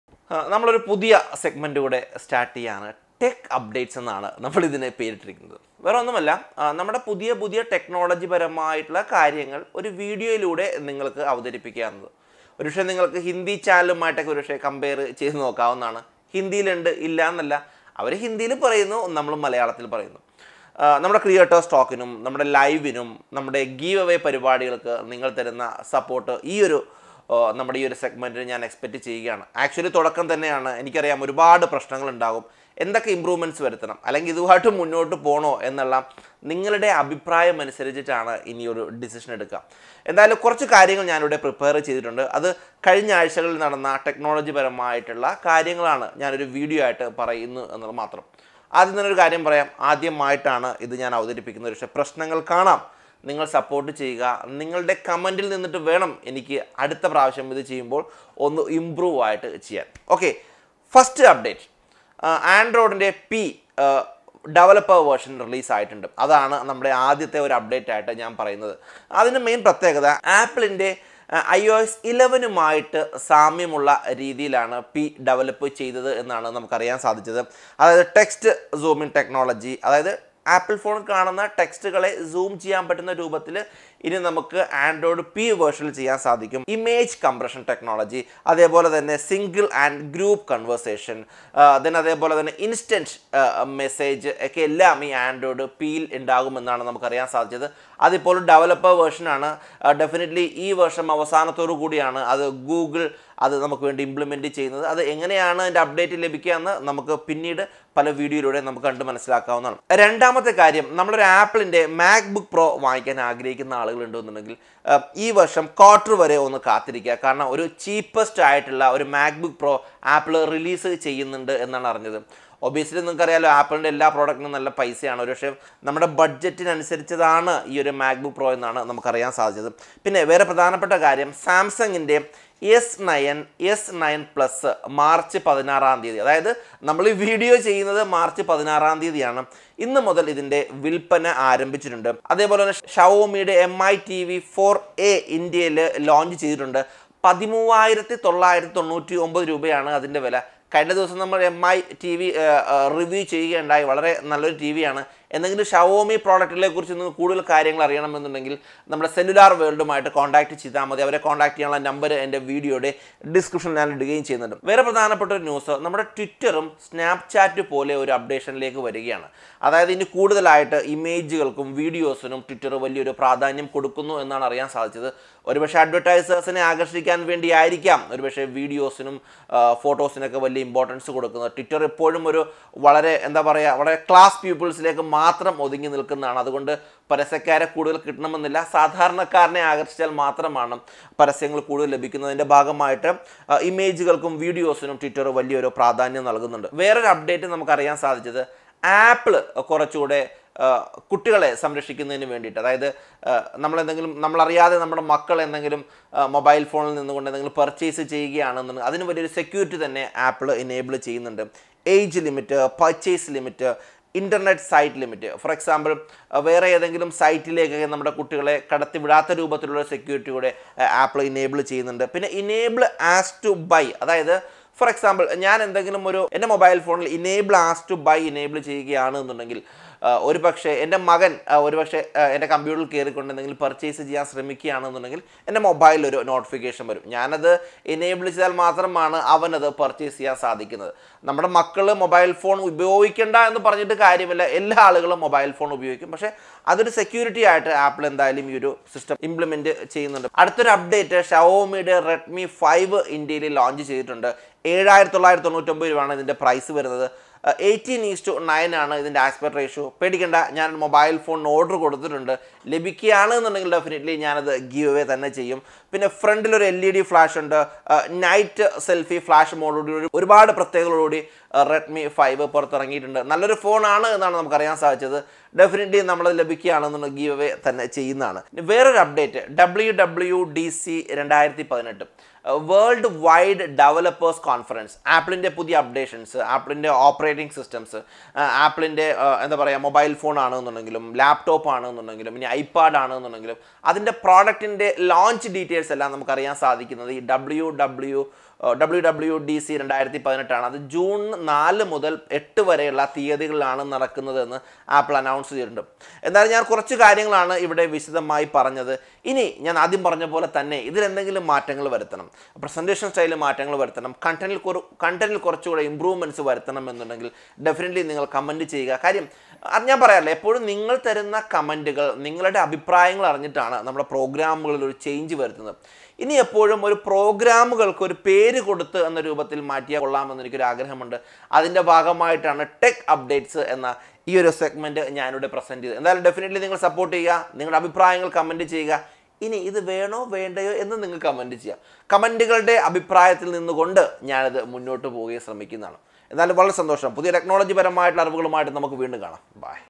😍പുതിയൊരു ഒരു പരീക്ഷണം - Tech Updates EP#1 - Android P, Mac Book Air, Redmi 5
Tech updates bring you latest tech news and tech information on every Friday in Malayalam. We love the most wonderful technology that can change the life.